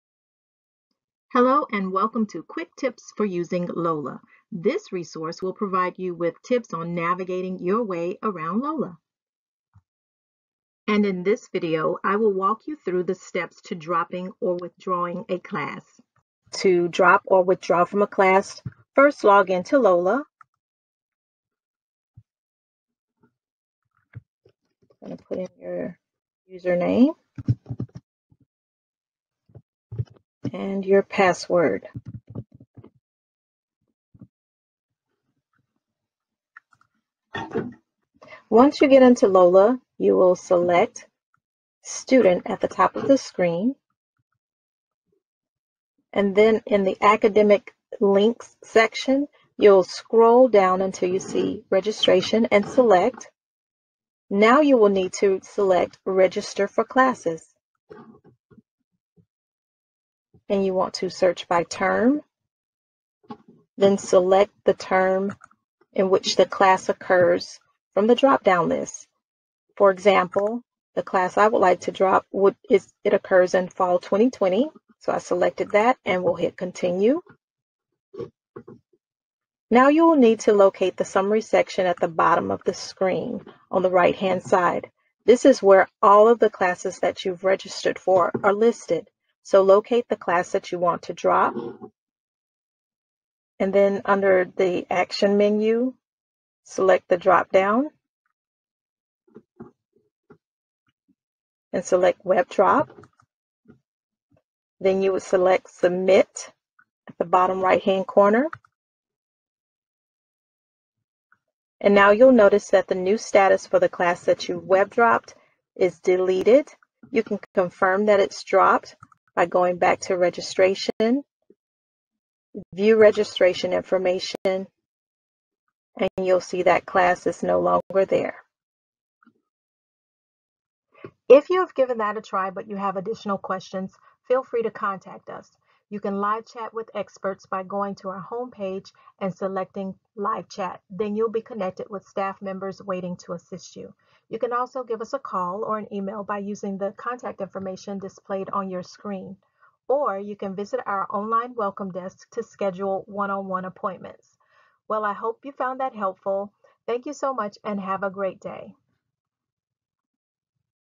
Delgado LoLA Navigation: Dropping a class
Navigate the new LoLA system with ease using these instructions to help you drop a class